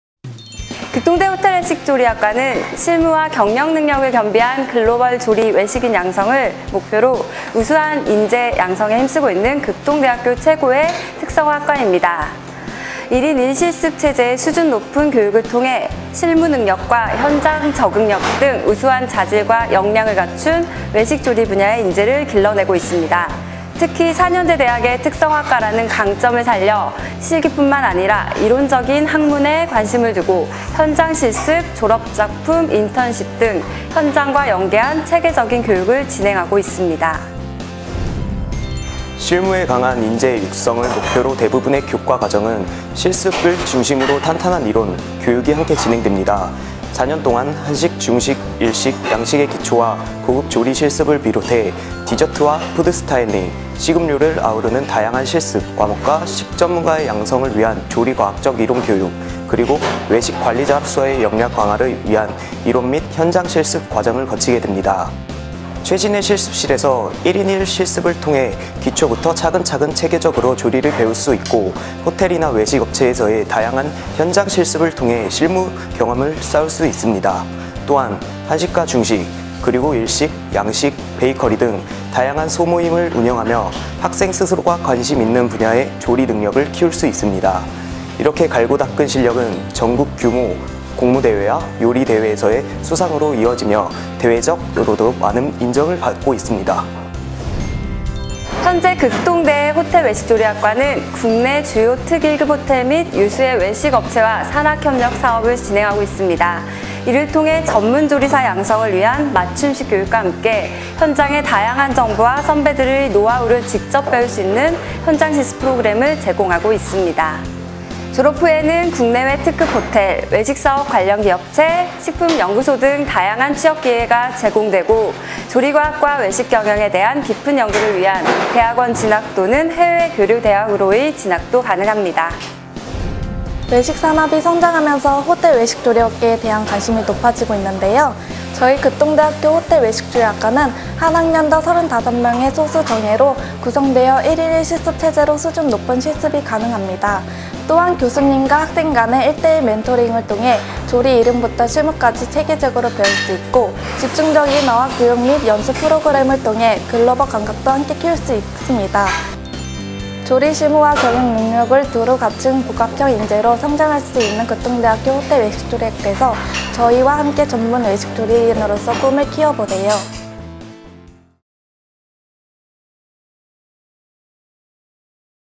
[하이틴TV] 극동대학교 - 호텔외식조리학과
Meet the World!
학생과 세상에 가치를 더해주는 강소대학 극동대학교
호텔외식조리학과를 소개합니다!